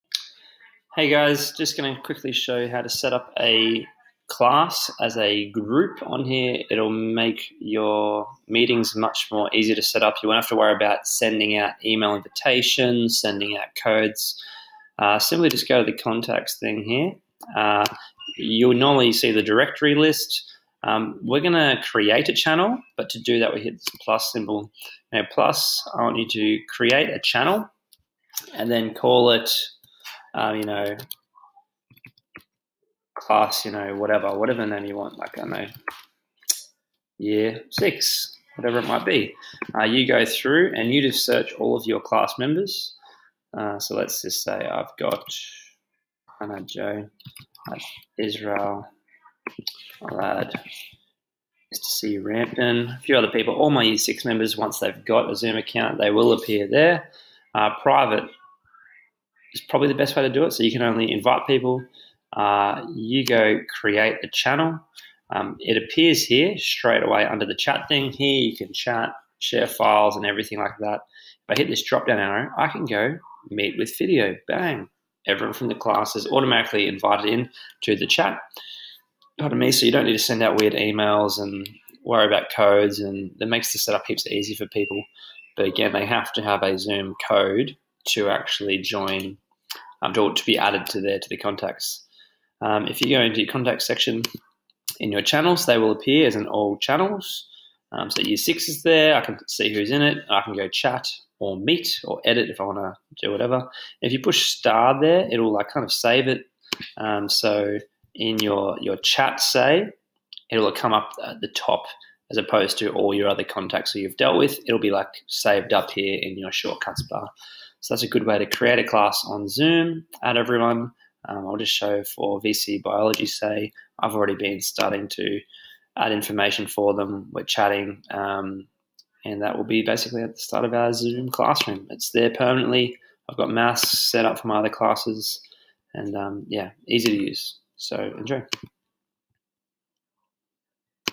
How to Setup Zoom Classrooms/Channels
Setting up Zoom Channels is a great idea when using Zoom as you can video call everyone with one click of a button, no invitations, no emails, no fuss. You also can maintain group conversation and file sharing throughout the day and week between sessions, making regular classes or sessions more connected.